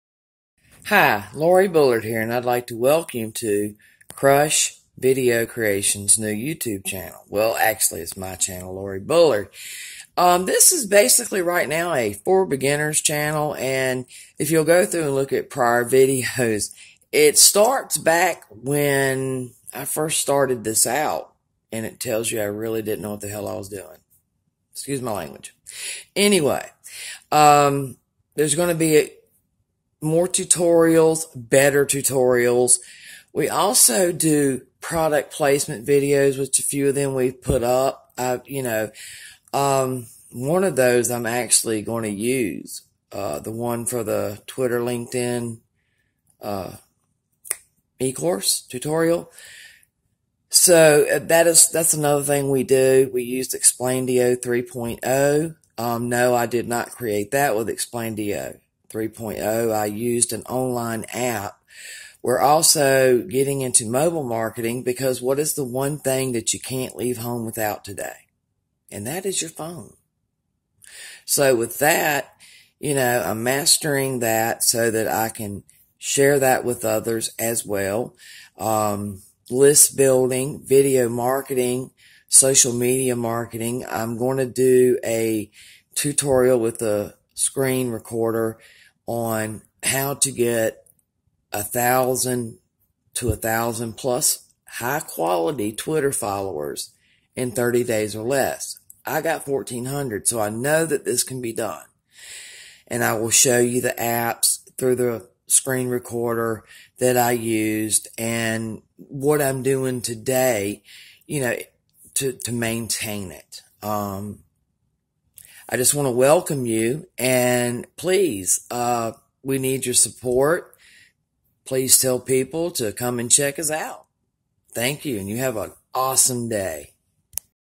Hello and Welcome to Laurie Bullard and KRUSH VIDEO CREATIONS
Information on what to expect from me and our various Marketing Tutorials and  Guests! Coming Soon... How​ to Get 1000+ Twitter Followers in 30 Days!